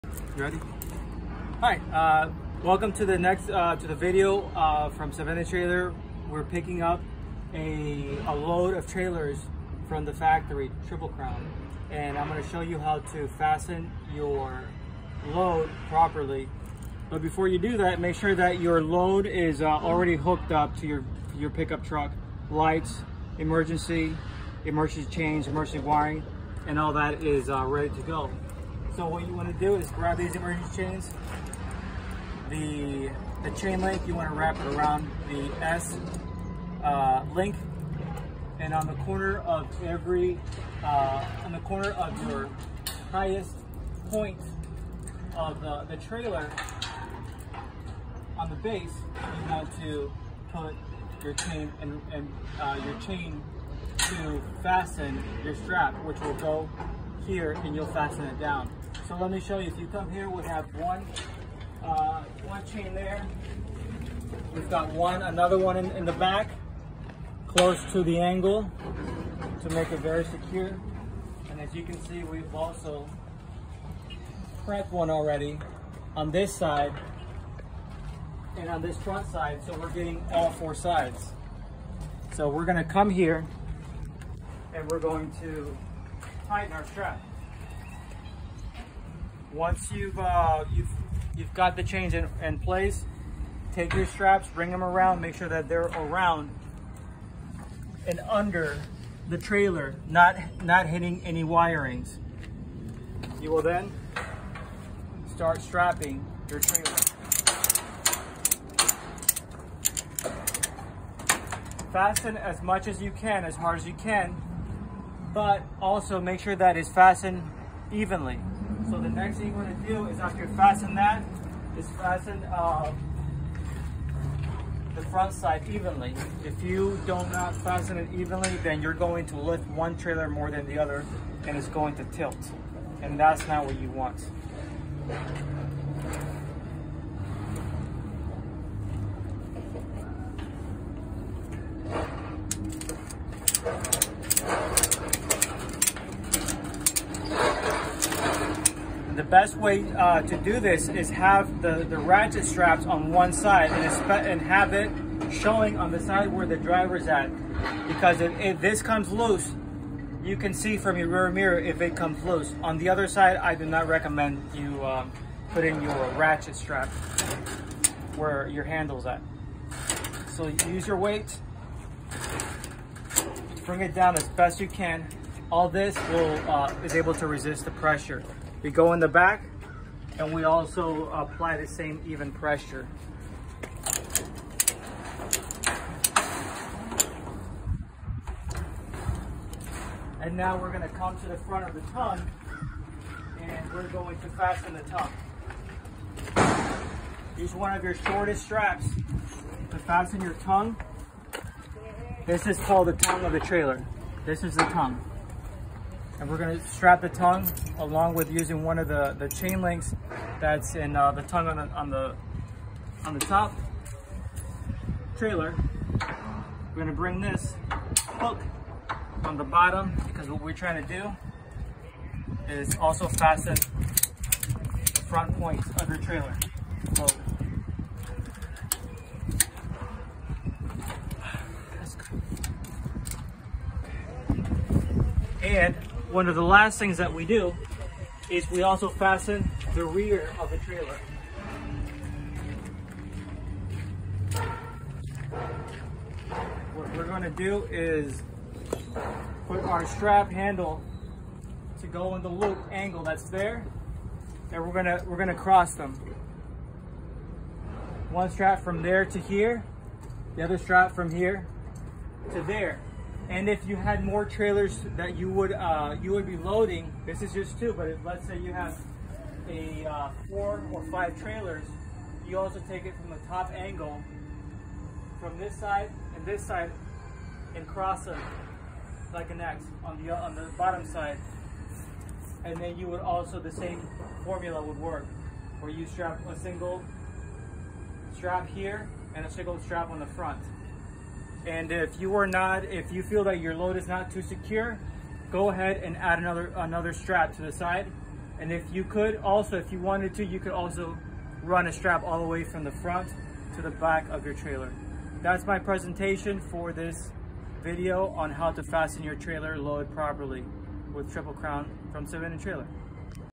Trailer training. How to fasten a stacked load of utility trailers for secure transport on the road
In this video I will show you how to properly fastened trailers that are stacked one on top of the other for easy and safe transportation on the road. I manage a utility an enclosed trailer business in Fort Myers Florida for many years. It is very important that if you work transporting trailers for us that you do it properly and safely. Tools you need to get the job done are four Safety chains six heavy duty straps and a little bit of muscle. In this video I am strapping two stacked 6 x 16 tandem axle trailers with 4 foot mesh that I am picking up from Triple Crown Trailers an Ocala Florida and haul them to my store with a pick up truck
. Please learn some thing and if this video is helpful pass it on.

Se Vende Trailer
Sevendetrailer.com

Video created by Marvin and Ralph